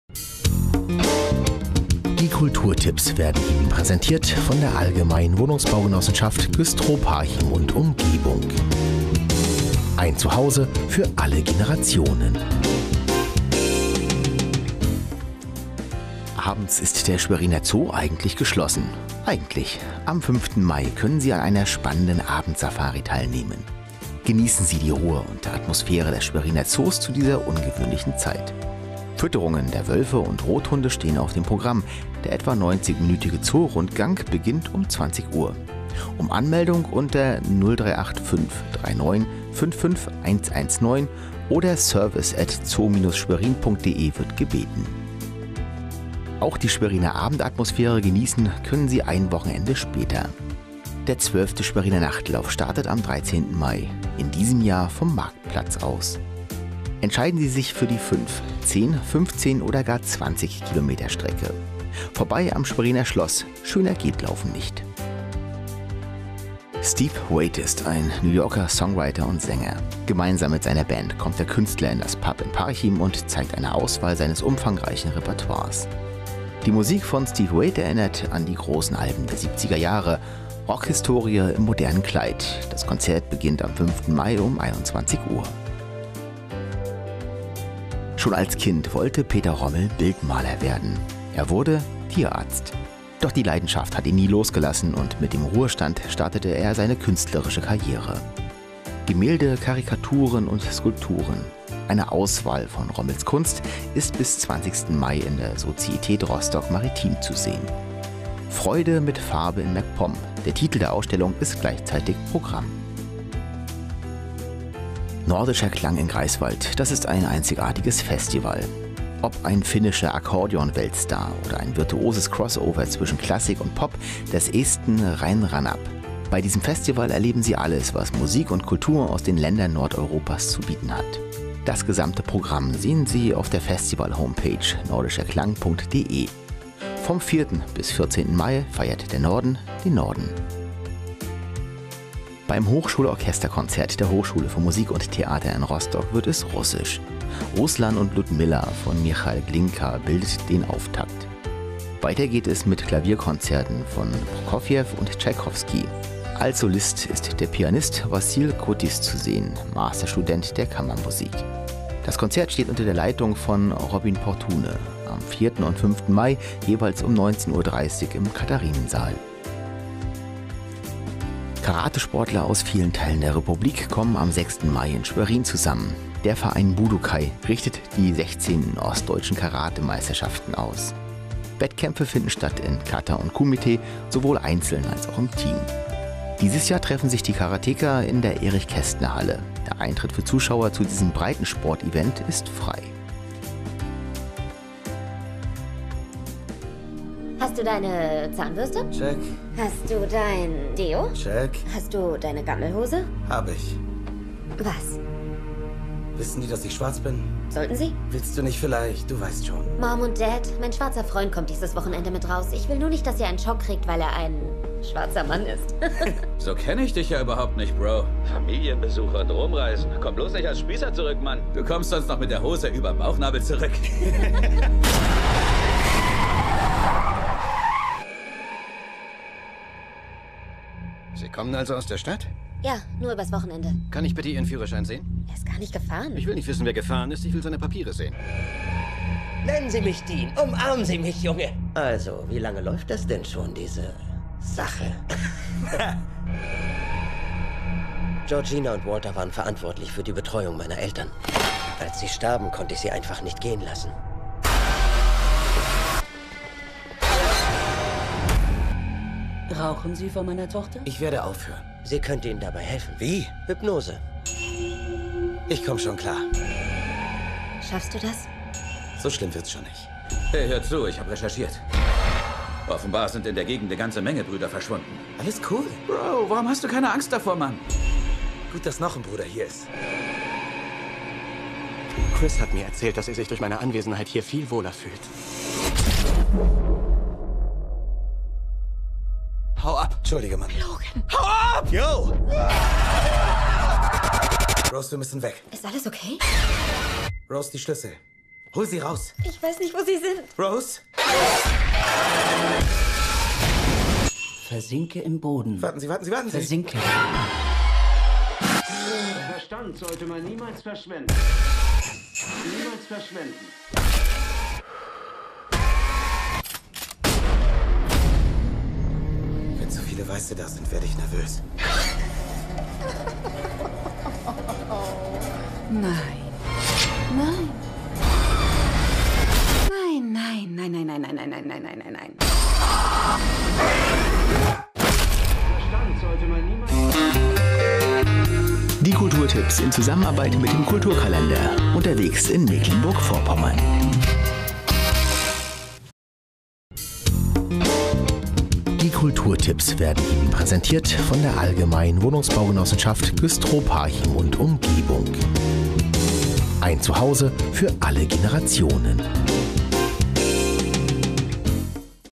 Veranstaltungshinweise
Was ist los in unserem Land?
Unter anderem:

Abendsafari - Zoo Schwerin
12. Schweriner Nachtlauf -  Start Marktplatz Schwerin
Steve Waitt Band - Irish Pub Parchim
Freude mit Farbe in Meck-Pomm -  Societät Rostock maritim
Nordischer Klang -  Greifswald, nordischerklang.de
Hochschulorchesterkonzert - Katharinensaal HMT Rostock
Ostdeutsche Karate Meistereschaften - Erich-Kästner-Halle Schwerin